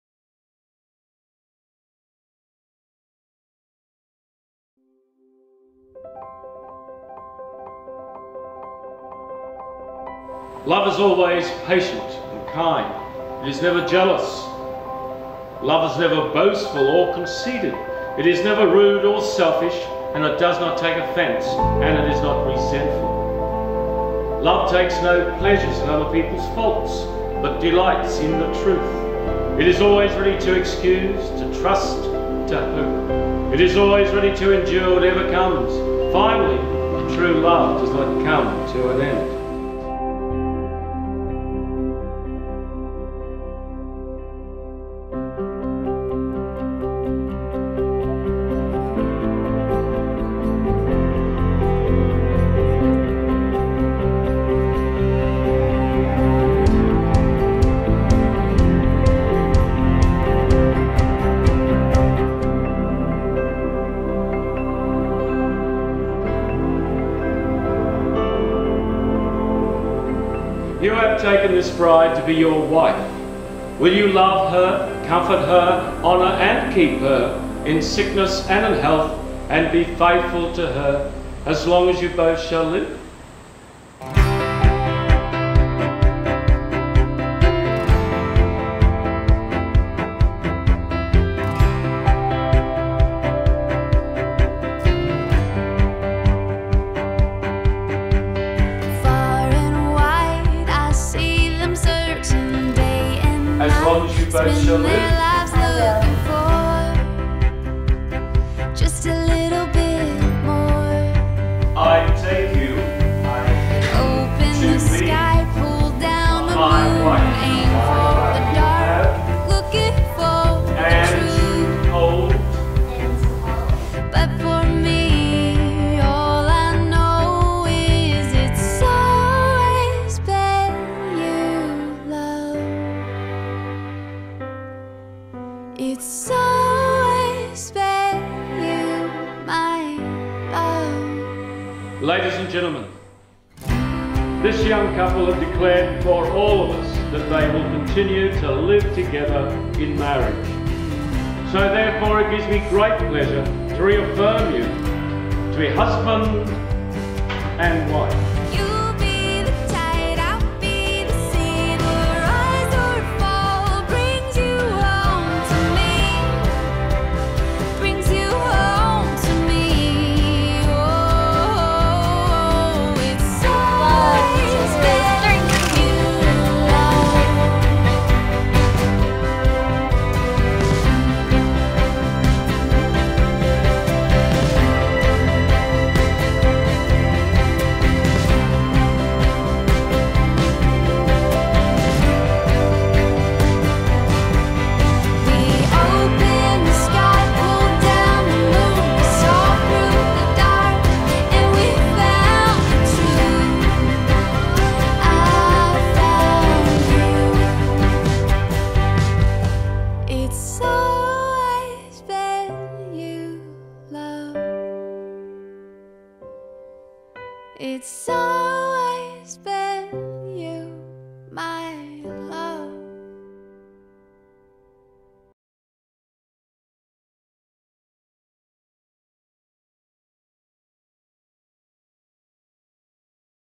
HILTON BLUE HORIZON CHAPEL
The Hilton Blue Horizon Chapel is an exclusive, purpose built, non-denominational wedding chapel situated on the stunning foreshore of the Cairns inlet in the grounds of the acclaimed Hilton Hotel Cairns.